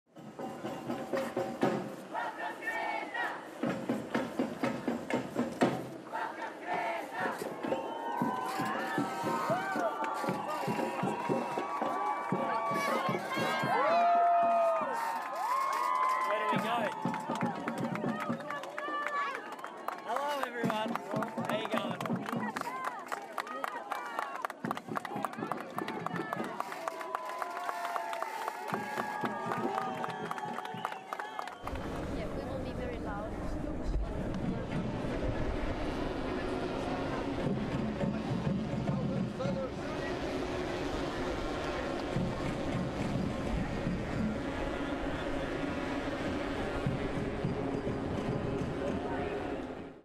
Greta Thunberg heading to COP25 in Madrid after crossing Atlantic on catamaran
Thunberg missed last Friday's climate strike as heavy winds delayed her arrival in Lisbon but she is due to join thousands of activists marching in Madrid on Friday.